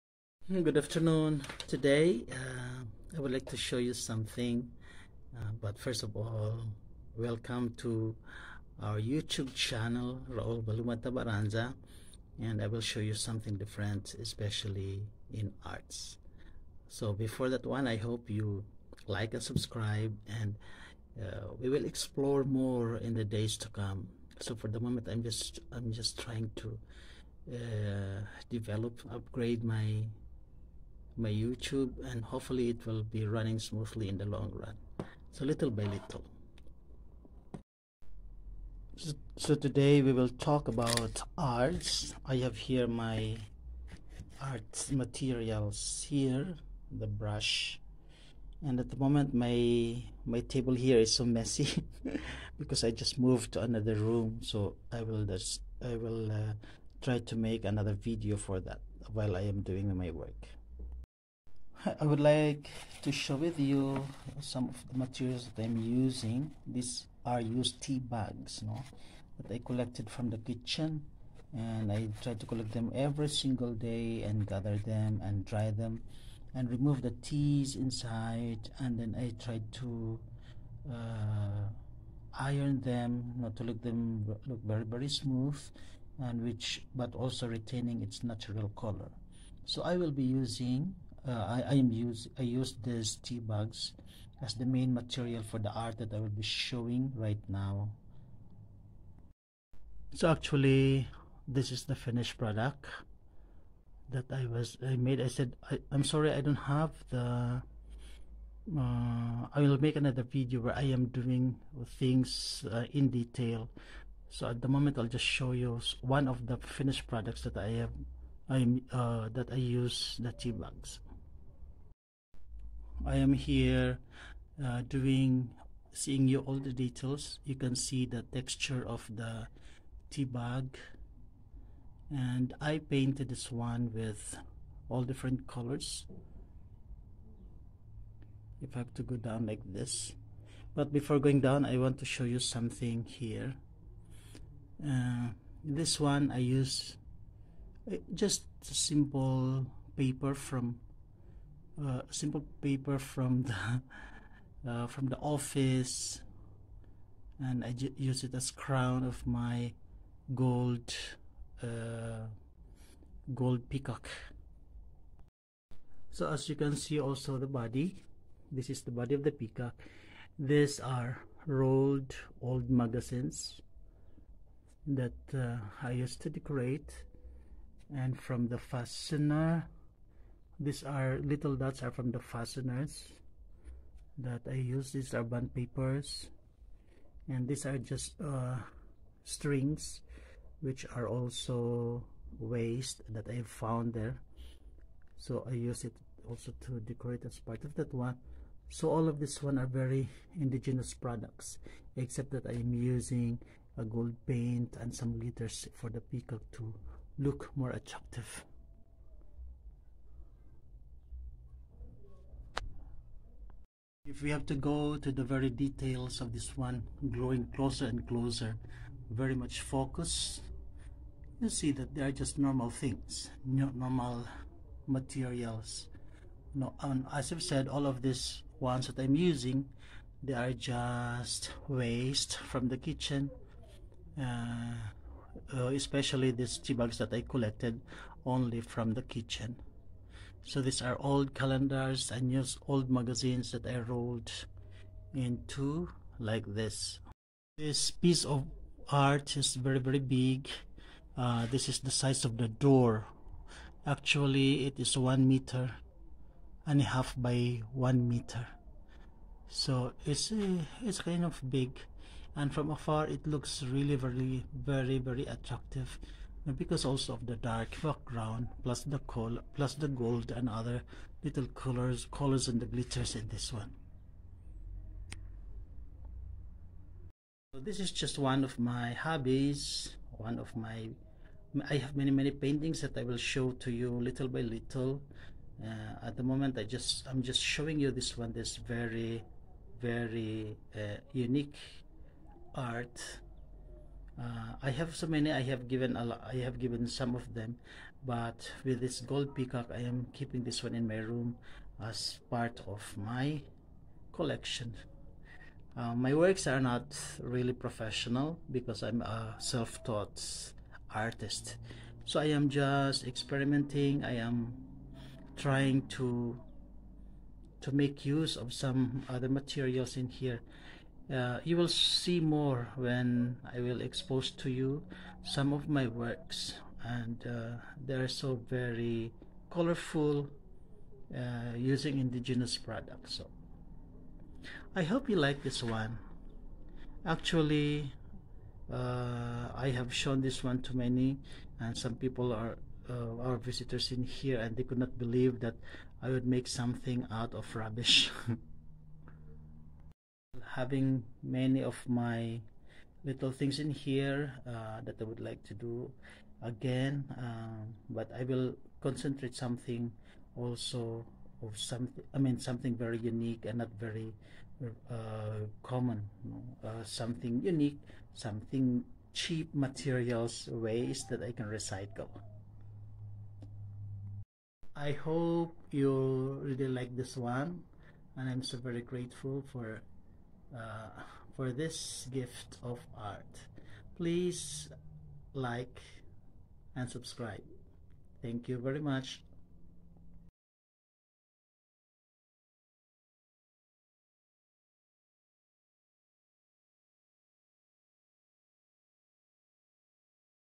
Art and Painting Tea bags- Golden Peacock
My unique piece of art using used tea bags, old magazines and pieces of strings that I found in our rubbish bin.